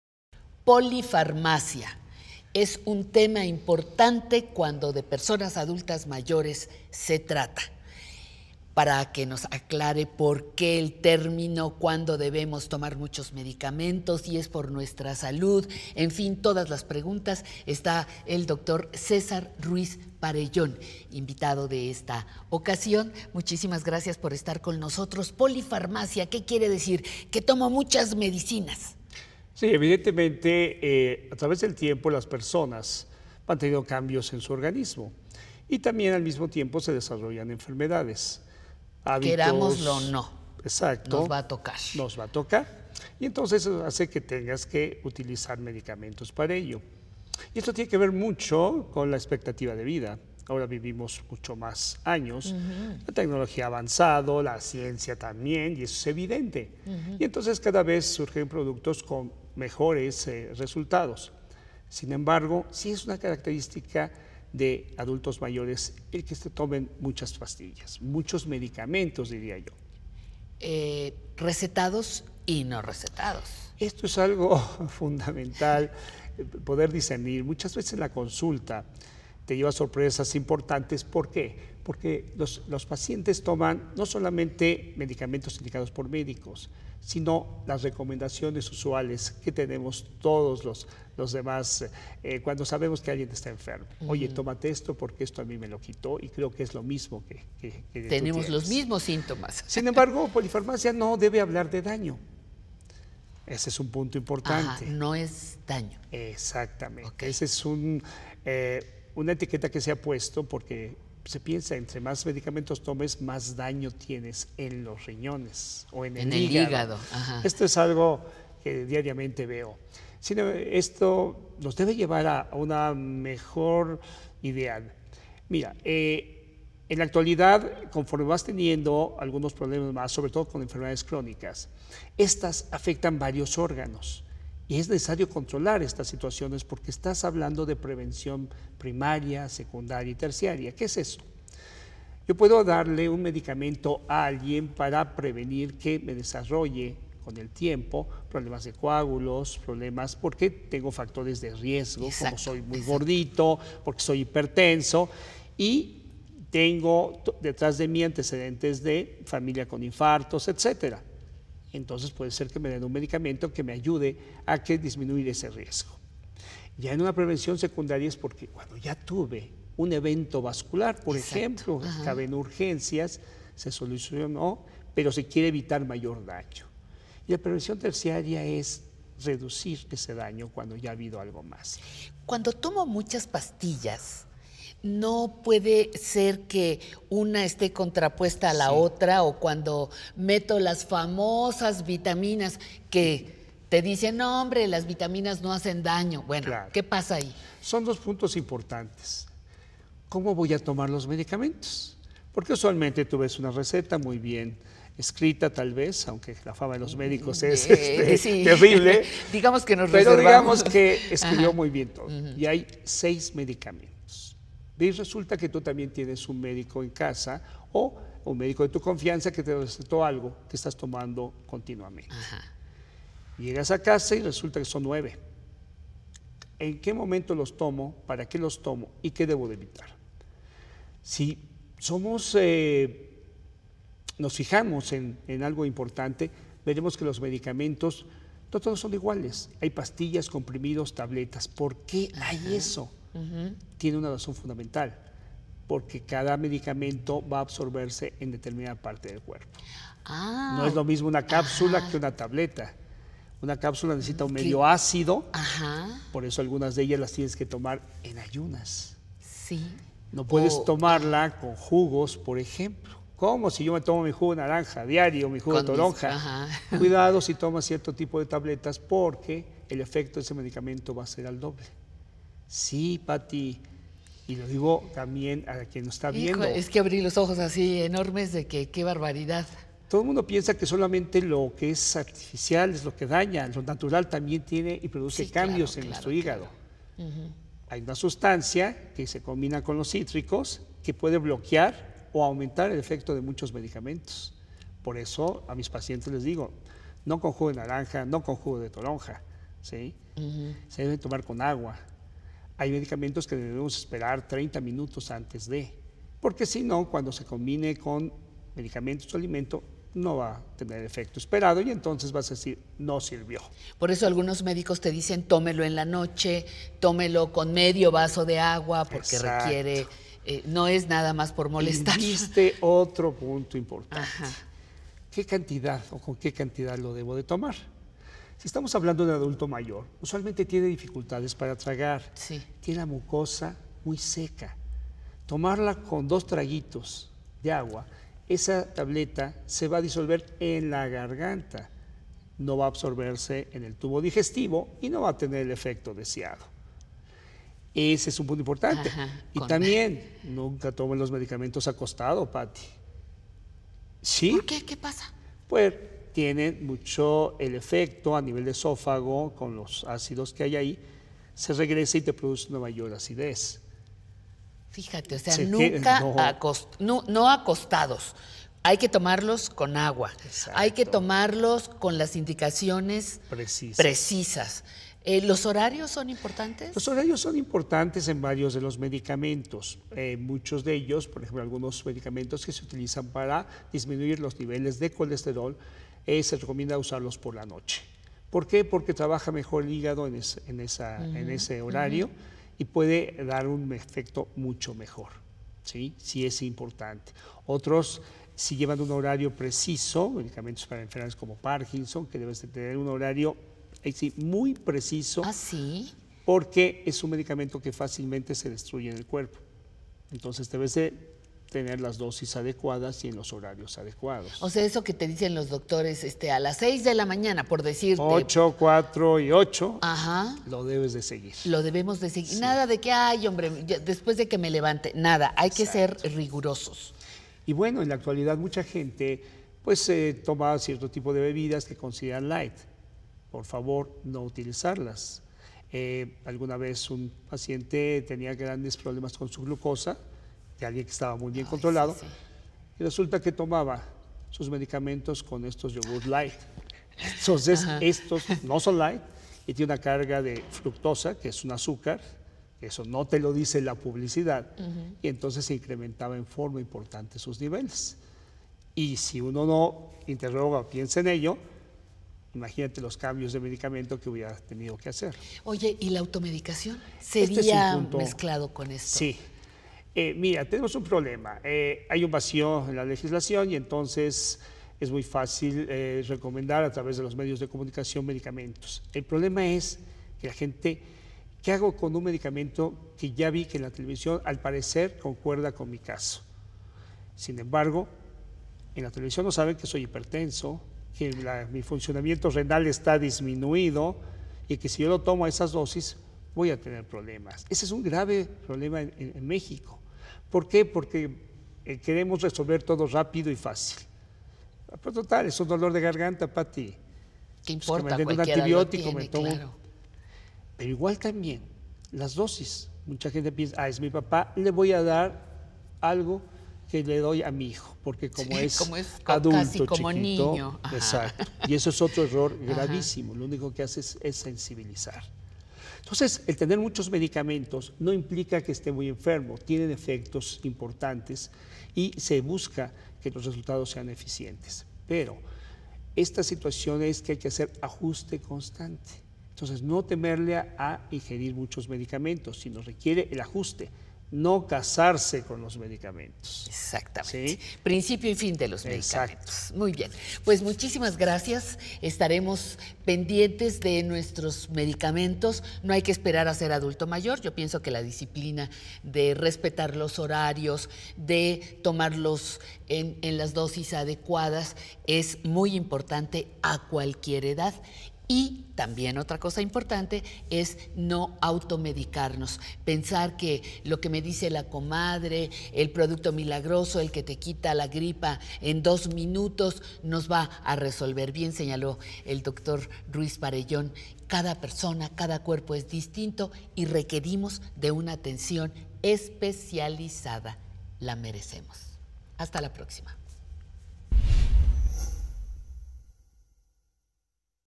¿Tomas medicamentos? Algunas recomendaciones para ti | Vejez creativa con Patricia Kelly
¿Te tomas el comprimido con un "tragito" de agua o con un vaso entero? ¿con jugo o con agua? ¿Por qué unas son pastillas y otras cápsulas? ¿qué efectos puedes generar tomar el medicamento en la mañana o por la noche? El doctor César Ruiz tiene las respuestas para ti. No te pierdas este video y compártelo con tus amigos, familiares y conocidos.